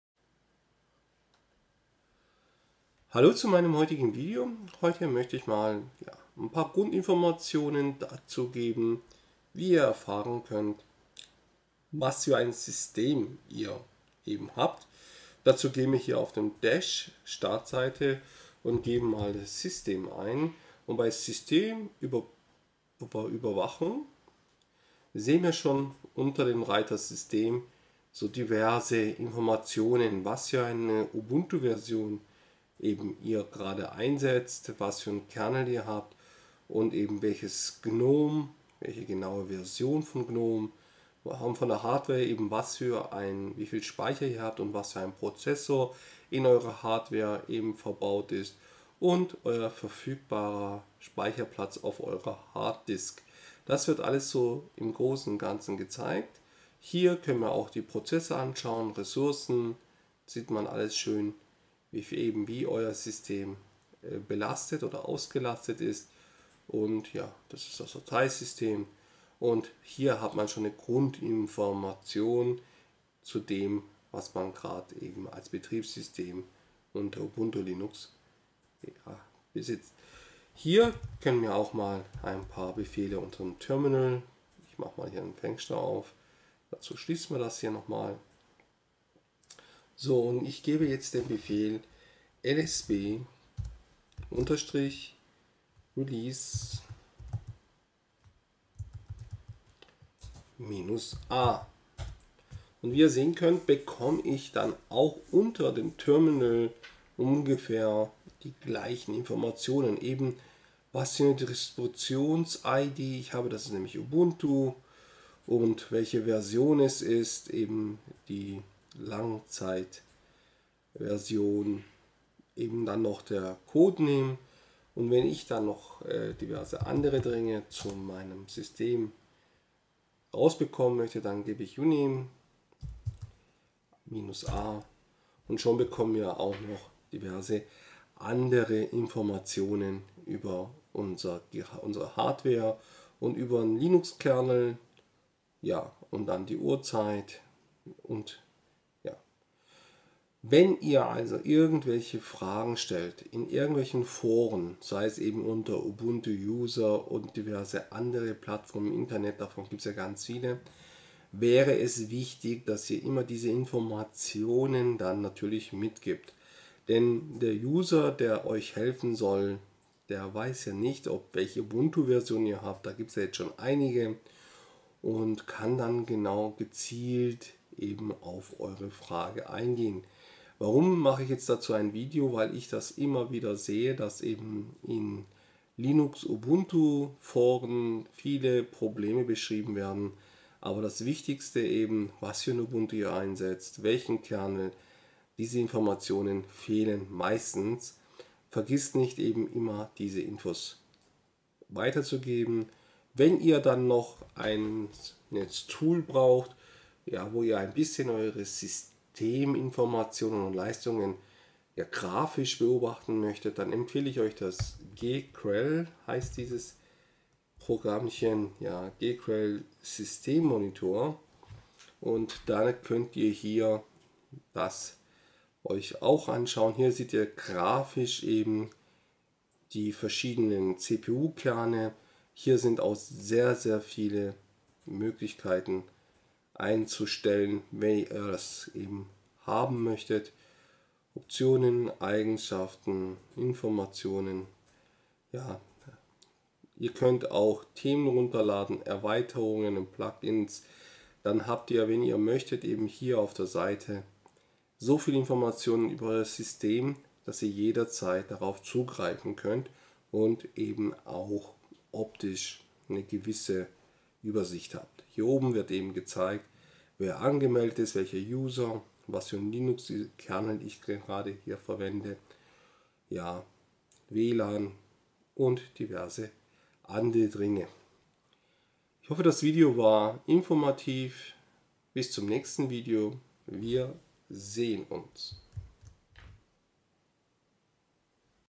System Informationen auslesen unter Ubuntu
Was für ein Linux habe ich und welcher Linux-Kernel ist gerade mein aktueller? Wie bekomme ich diese Infos. Genau zu diesen Fragen habe ich ein Video vorbereitet. Viel Spaß dabei!